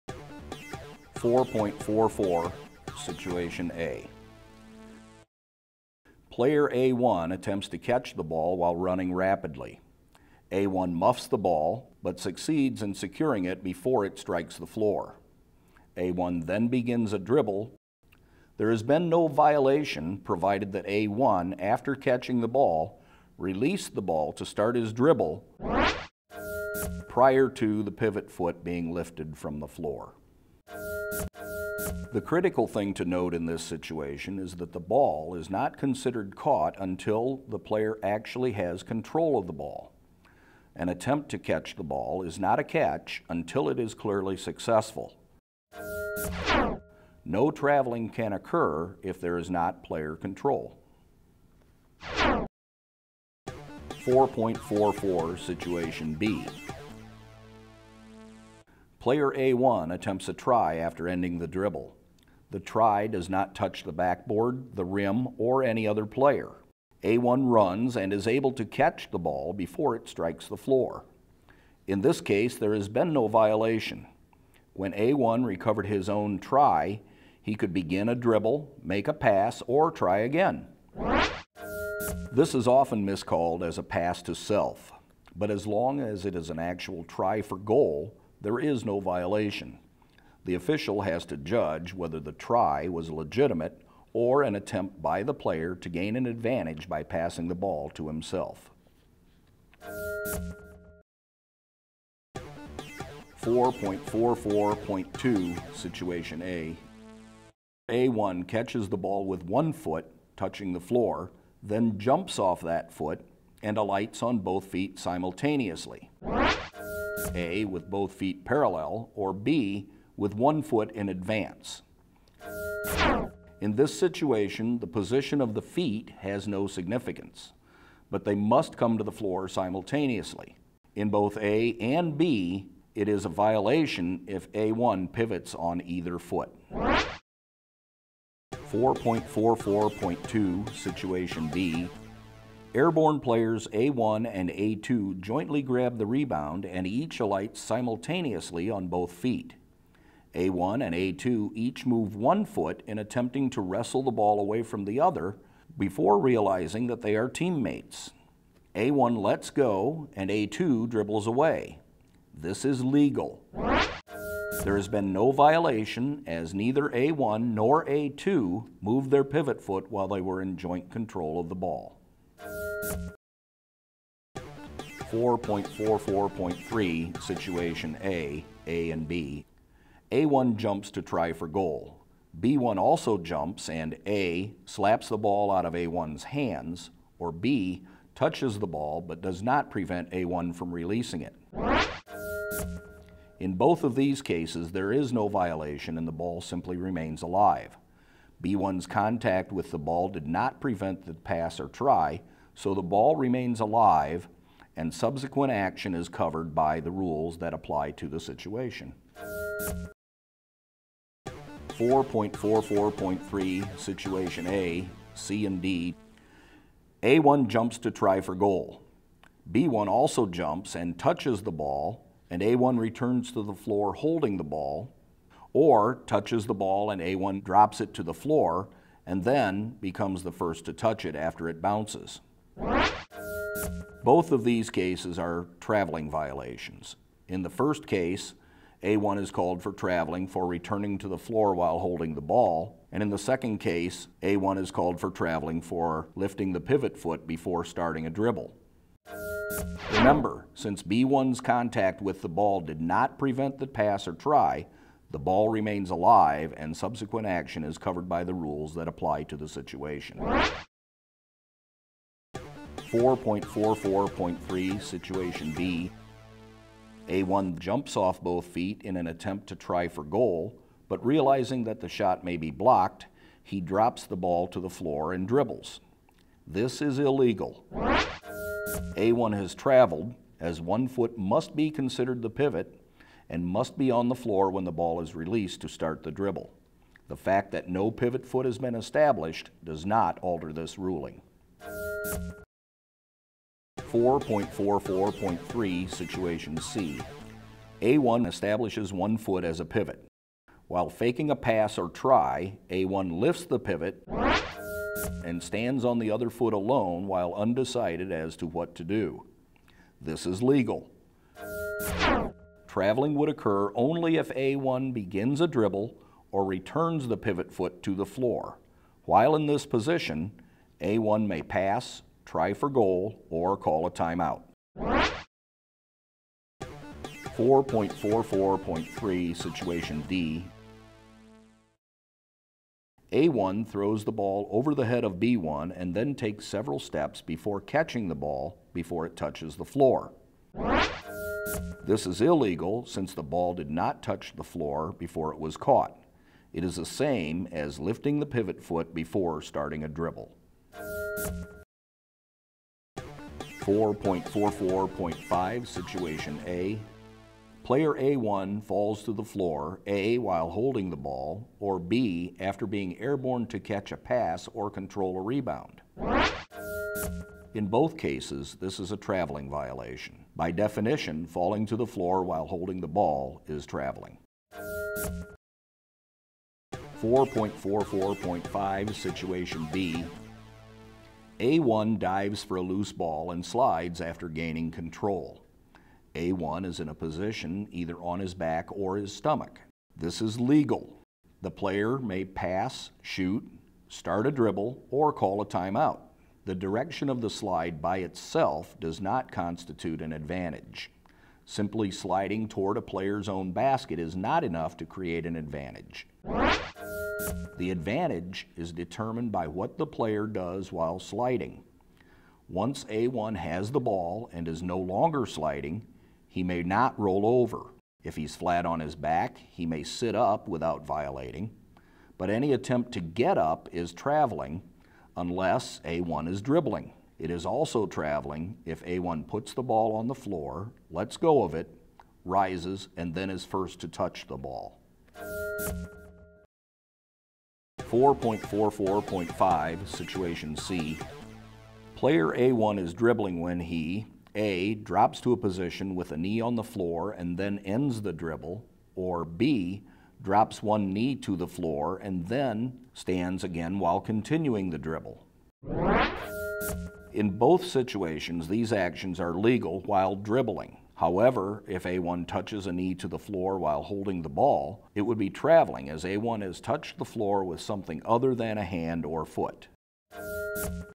OSHAA Officials Education Volume 1: Traveling
Traveling (Rule 4-44) is one of the most often missed calls in basketball.  Many officials call traveling because a player's foot movement "looks funny."  This reflects a lack of thorough knowledge of the rules and does a disservice to players and their teams.  Officials should strive to get the call right by rule instead of relying on instinct or what they may have learned during their days as a player or coach.  This DVD will help officials at all levels make the right calls.

This video was produced by the BGSU School of Art, Digital Arts Division, Spring 2011.
Heather Elliott-Famularo, producer.
Scott Foor, Consultant